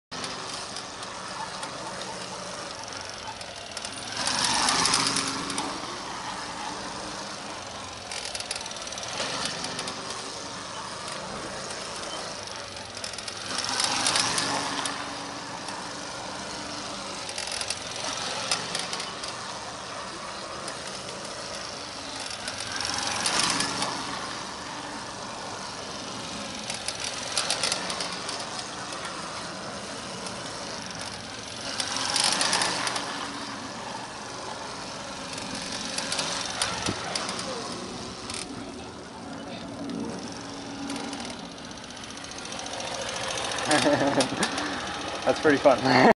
110cc knee down figure 8's at XRC Bangkok
having fun on mini bikes... pulled out after losing the front